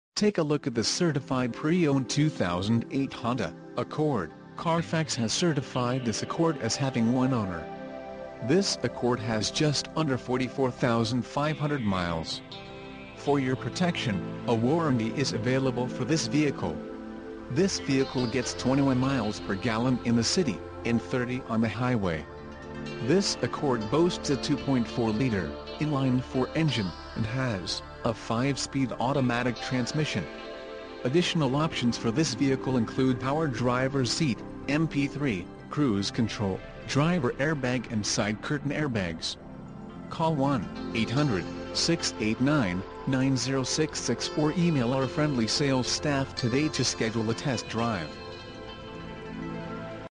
2008 Honda Accord LX-P Sedan Sedan - Franklin, TN
Darrell Waltrip Pre-Owned, Franklin, TN - ABS BRAKES, CRUISE CONTROL, POWER DRIVERS SEAT